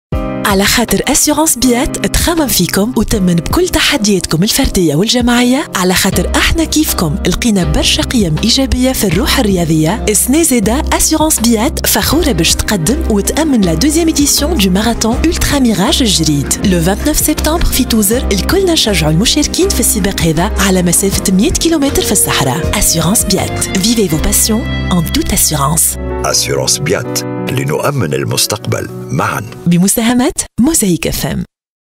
🎙Assurances Biat présentent Ultra Mirage El Djerid 100km 2018 📻Spot Radio Mosaïque Fm🇹🇳
🇹🇳Assurances BIAT présentent Ultra Mirage El Djerid 100 km - 2e édition 29-30 septembre 2018 à Tozeur
Ultra Mirage® est un Ultra trail International de 100 km en moins de 20 heures.

Agence Hapax© | Radio Mosaique FM© | Ultra Mirage®